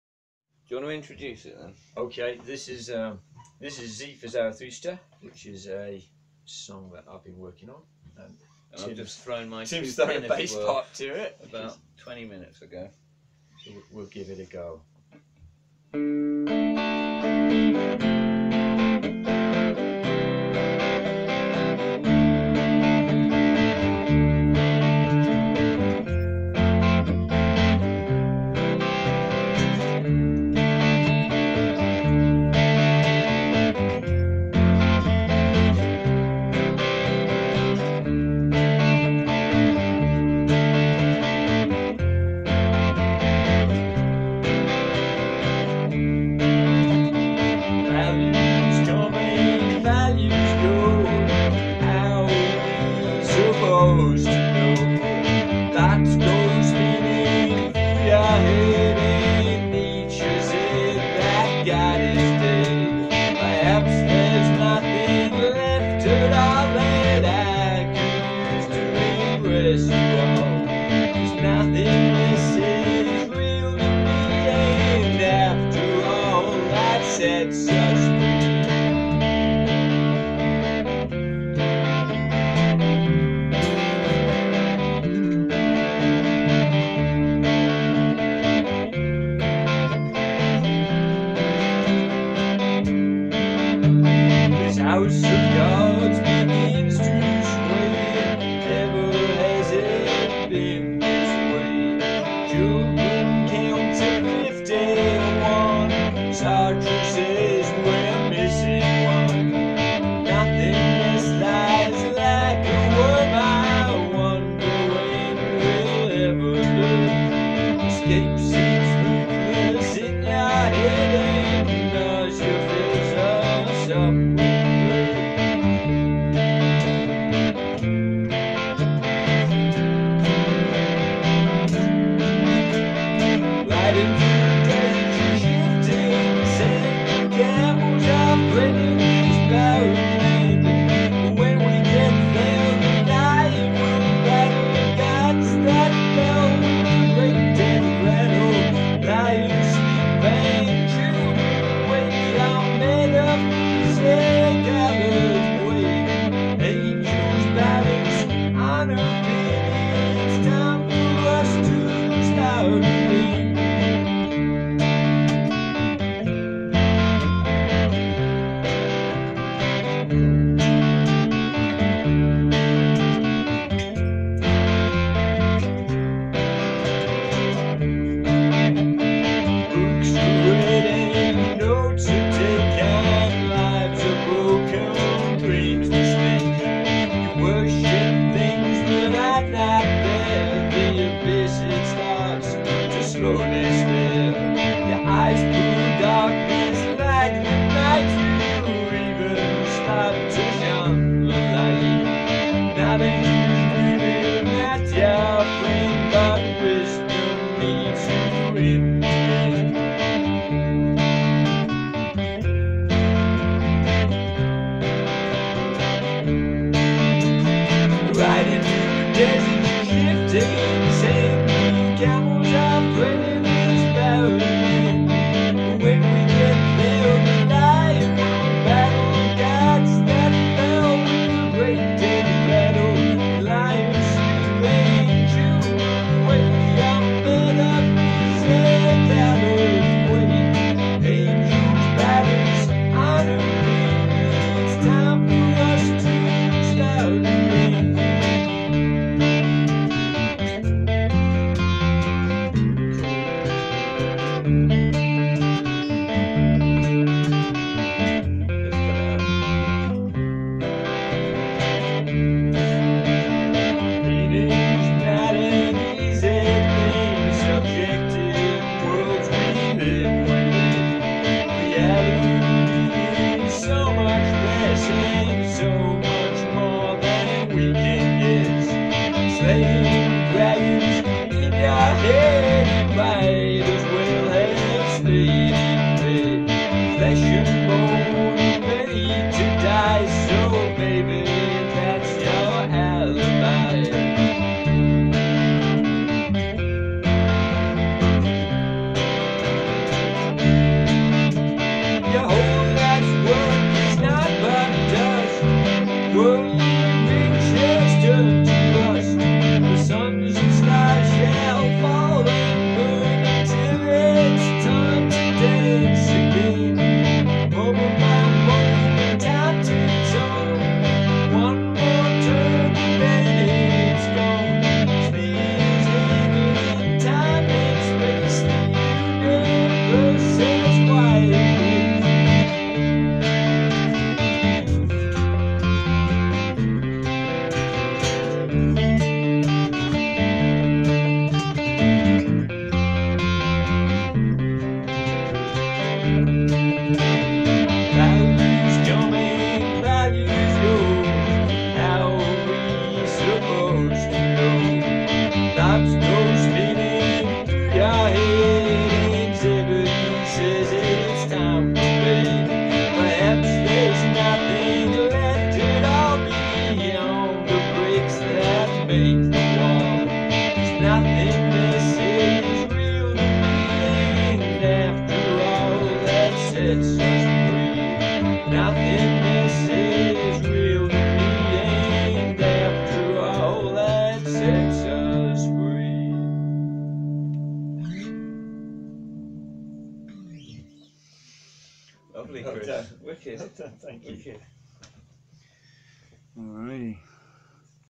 Pre-Halloween Jam /// BLACKTOOTH + THE HODGE...
So... Chris Hodgson visits Cawley road Fox's Den, to Jam with me. Chris is using a  Gretsch G5122 to good effect, I'm on the old 80's Korean precision bass. No mic on vocals; we're capturing a moment, having run through it for the first time twenty minutes prior... Anyway, bless ya Chris for manning up & performing for my camera.